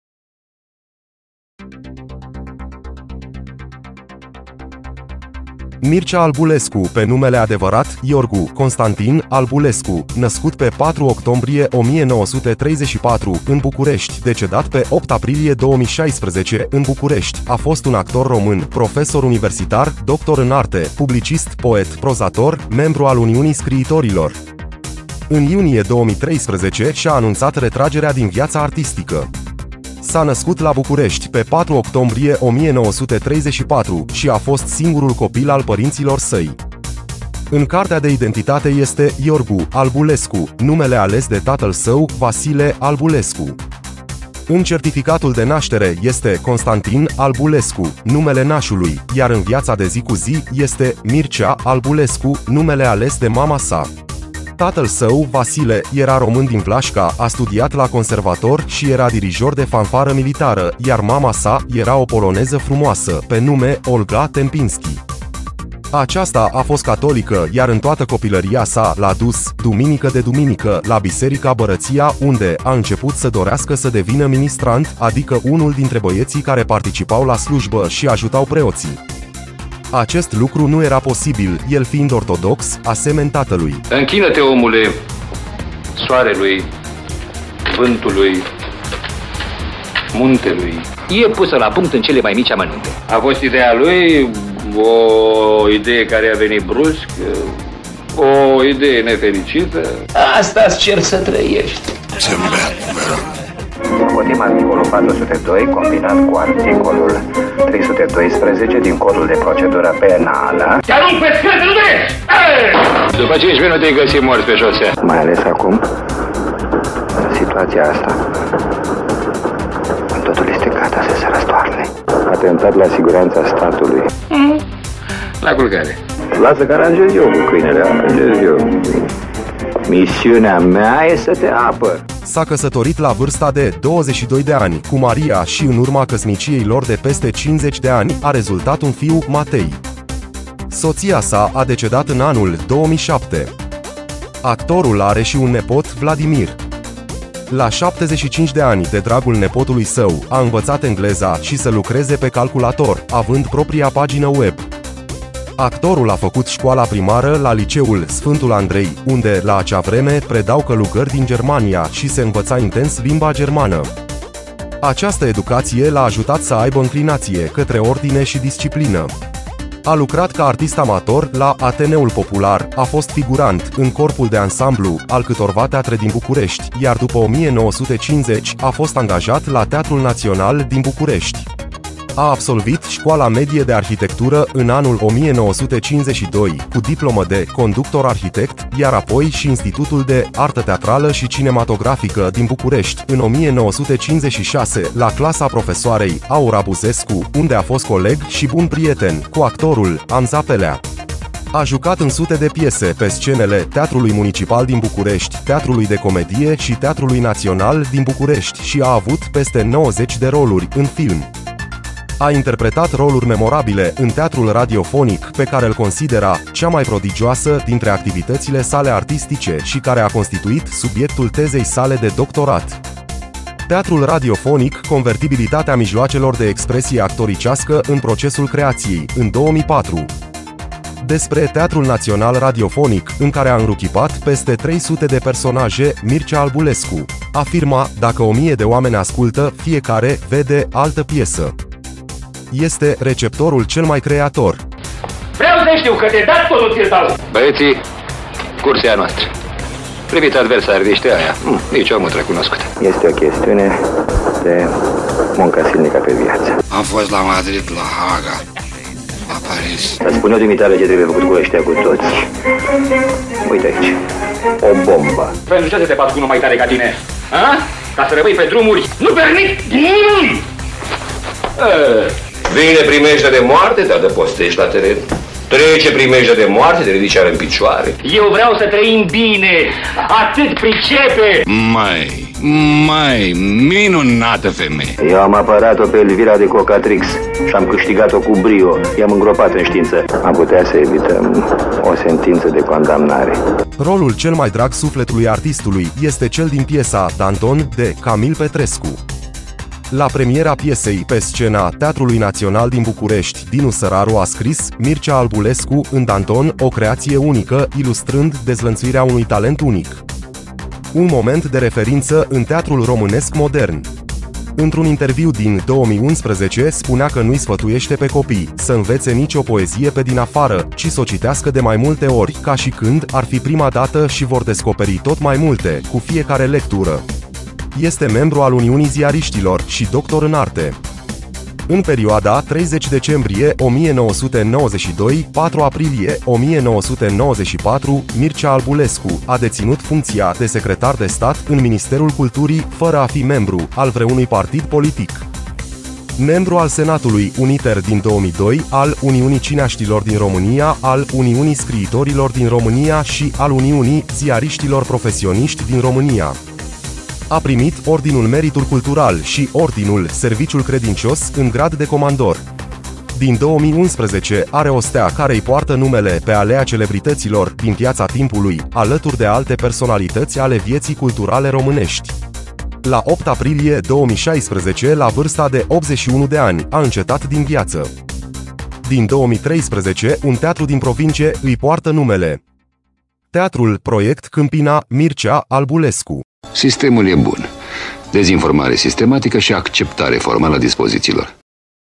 Cei ce nu mai sunt. Mircea Albulescu.
Mircea Albulescu a fost un actor român, profesor universitar, publicist, poet, prozator, membru al Uniunii Scriitorilor.